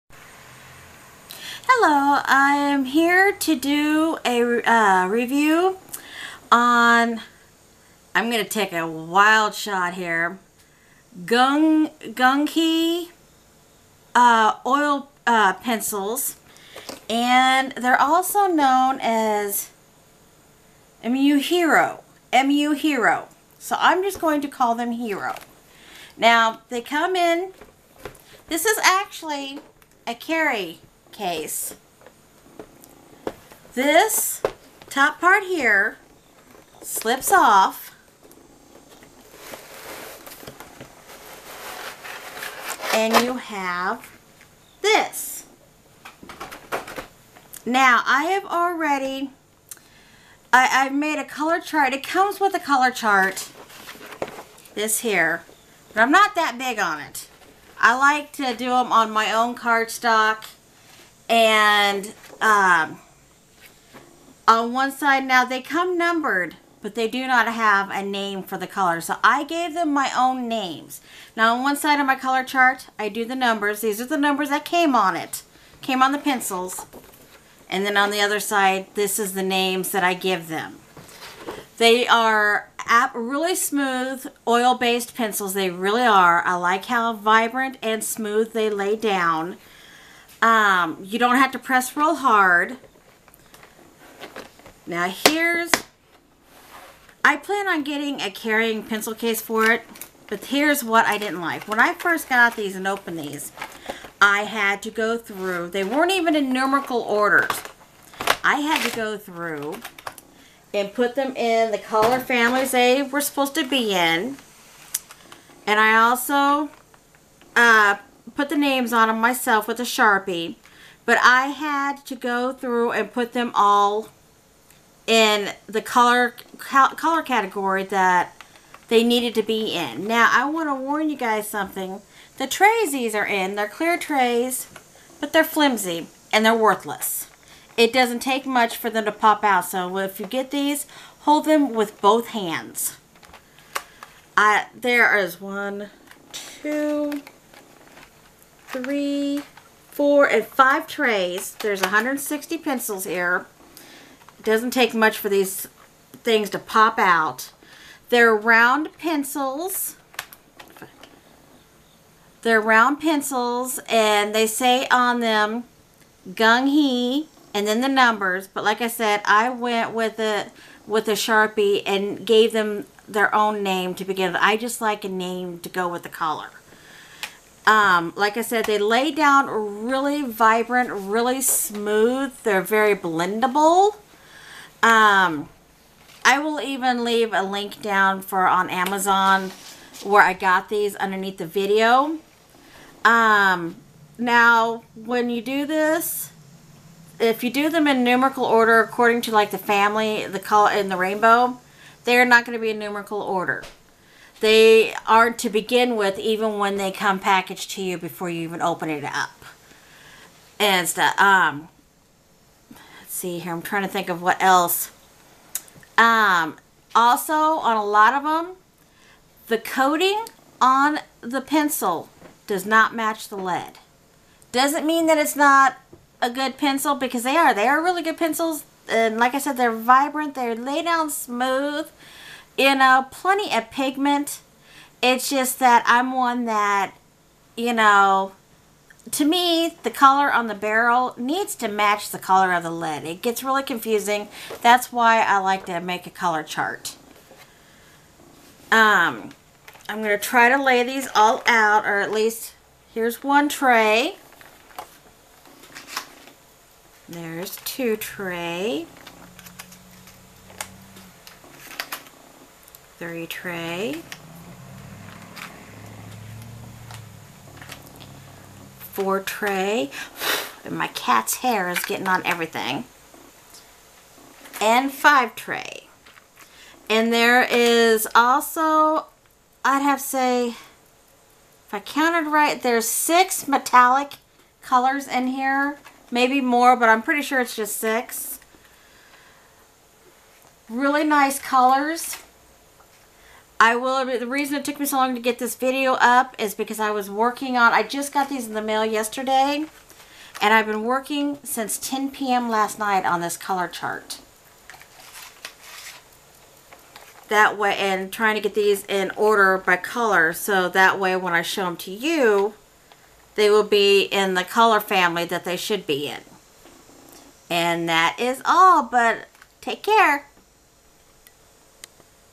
Hero oil pencils 160 pack review by Tamara Farley
Hero 160 Oil Colored Pencils -Classic & Professionals - Assorted Colors for Sketch Coloring Pages For Kids & Adults - Vibrant Colors For Drawing Books - Set of 160

Dollardiva also made a AWESOME video on these same pencils comparing how they worked on other paper, & also compared them to Prisma's, you will be surprised at the results, check it out at this